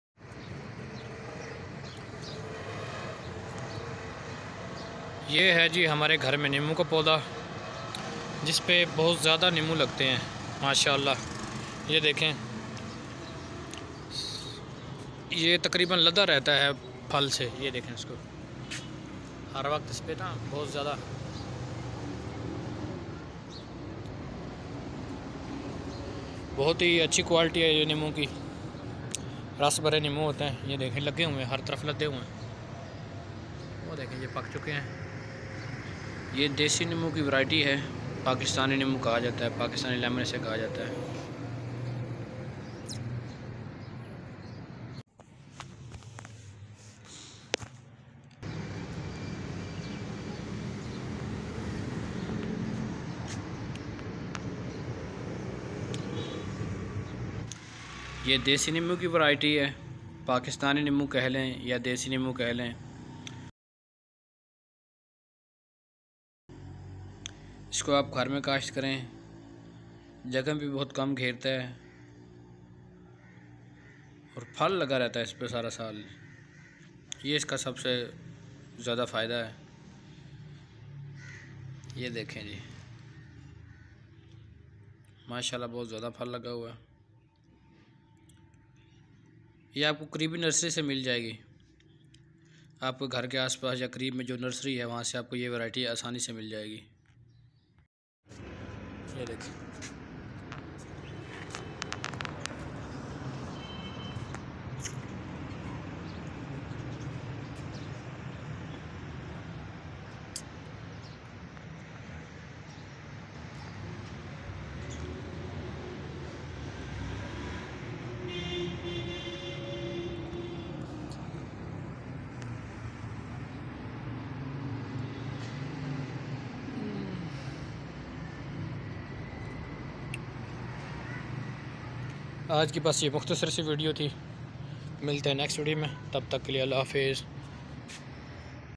Pakistani Lemon Plant Desilemonplant Lemonfarming Best lemon plant of world Pakistani lemon
This is a Pakistani lemon plant in my home